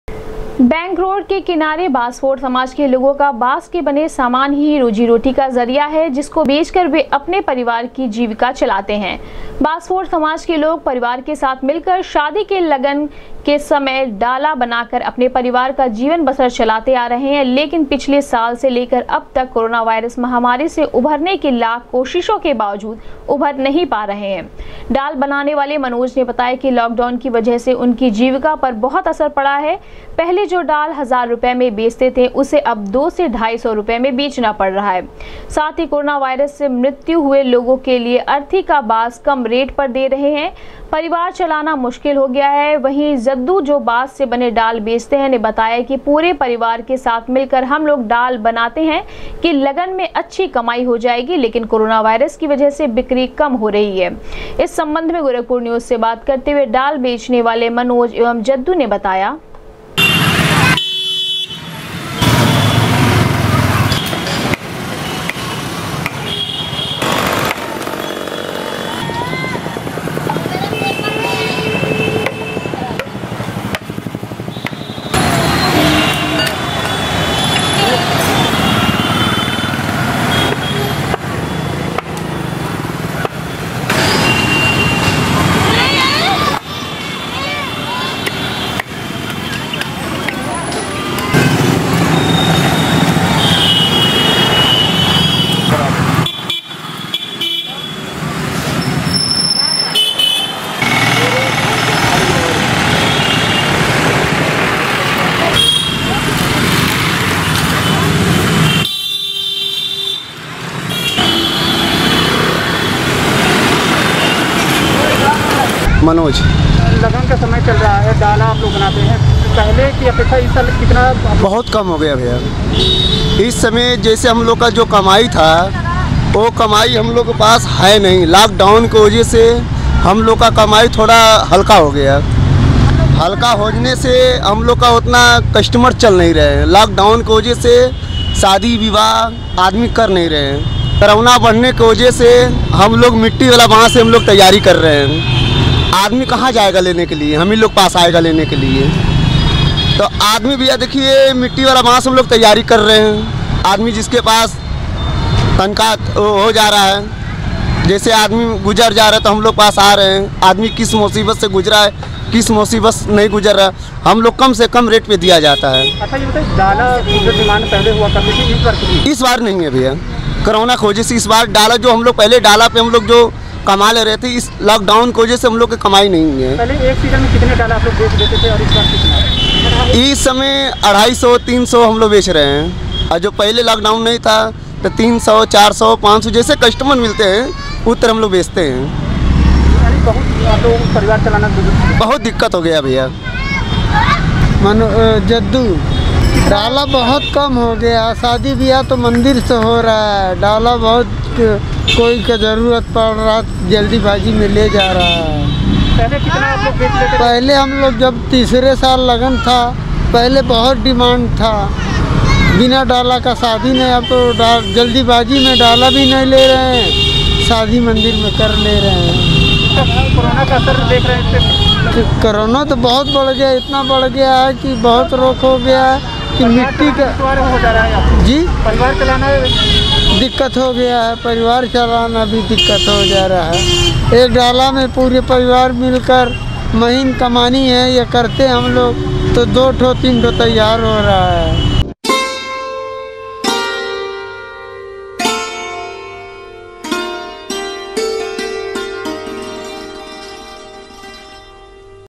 लाकडाउन से शादी का डाल बेचने वाले हुए मायूस, एक हजार रुपए में बिकने वाले डाल को बेच रहे हैं दो से
लाकडाउन से शादी का डाल बेचने वाले हुए मायूस, एक हजार रुपए में बिकने वाले डाल को बेच रहे हैं दो से ढाई सौ में, कोरोना से मृत्यु हुए लोगों के लिए अर्थी का बांस दे रहे हैं कम रेट पर